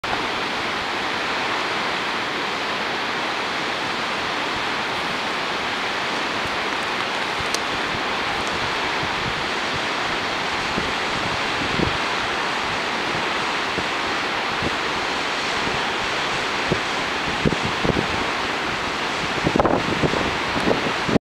Top of Vernal Falls
View of the top of Vernal Falls in Yosemite National Park